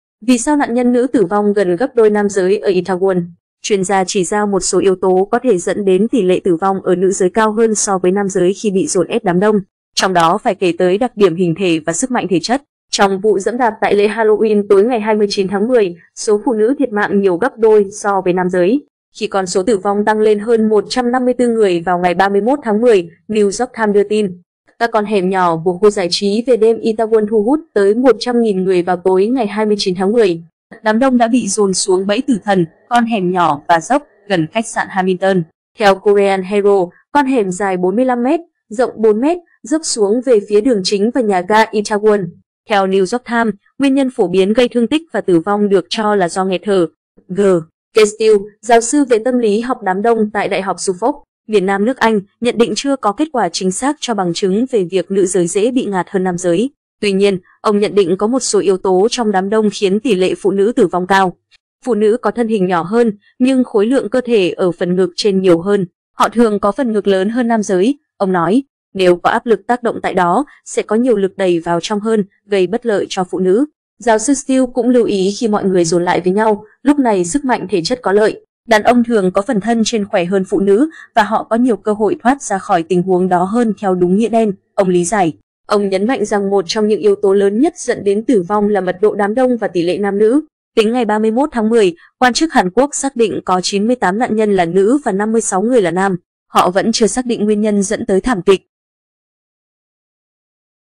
Vì sao nạn nhân nữ tử vong gần gấp đôi nam giới ở Itaewon
Chuyên gia chỉ ra một số yếu tố có thể dẫn đến tỷ lệ tử vong ở nữ giới cao hơn so với nam giới khi bị dồn ép đám đông, trong đó phải kể tới đặc điểm hình thể và sức mạnh thể chất.“Phụ nữ có thân hình nhỏ hơn, nhưng khối lượng cơ thể ở phần ngực trên nhiều hơn. Họ thường có phần ngực lớn hơn nam giới”, ông nói. “Nếu có áp lực tác động tại đó, sẽ có nhiều lực đẩy vào trong hơn, gây bất lợi cho phụ nữ”.
 Chuyên gia cho rằng các vụ giẫm đạp như thảm họa tại Itaewon lần này có thể xảy ra khi quá nhiều người bị nhồi vào một không gian kín. Ảnh: Yonhap.
Ông nhấn mạnh rằng một trong những yếu tố lớn nhất dẫn đến tử vong là mật độ đám đông và tỷ lệ nam nữ.
Giáo sư Still cũng lưu ý khi mọi người dồn lại với nhau, lúc này sức mạnh thể chất có lợi. “Đàn ông thường có phần thân trên khỏe hơn phụ nữ, và họ có nhiều cơ hội thoát ra khỏi tình huống đó hơn theo đúng nghĩa đen”, ông lý giải.
Tính ngày 31/10, quan chức Hàn Quốc xác định có 98 nạn nhân là nữ và 56 người là nam. Họ vẫn chưa xác định nguyên nhân dẫn tới thảm kịch.
G. Keith Still - giáo sư về tâm lý học đám đông tại Đại học Suffolk, miền Nam nước Anh - nhận định chưa có kết quả chính xác cho bằng chứng về việc nữ giới dễ bị ngạt hơn nam giới. Tuy nhiên, ông nhận định có một số yếu tố trong đám đông khiến tỷ lệ phụ nữ tử vong cao.
Các con hẻm nhỏ của khu giải trí về đêm Itaewon thu hút tới 100.000 người vào tối 29/10. Đám đông đã bị dồn xuống “bẫy tử thần” - con hẻm nhỏ và dốc - gần khách sạn Hamilton. Theo Korea Herald, con hẻm dài 45 m, rộng 4 m, dốc xuống về phía đường chính và nhà ga Itaewon.
Trong vụ giẫm đạp tại lễ Halloween tối 29/10, số phụ nữ thiệt mạng nhiều gấp đôi so với nam giới, khi con số tử vong tăng lên hơn 154 người vào ngày 31/10, New York Times đưa tin.
Theo New York Times, nguyên nhân phổ biến gây thương tích và tử vong được cho là do nghẹt thở.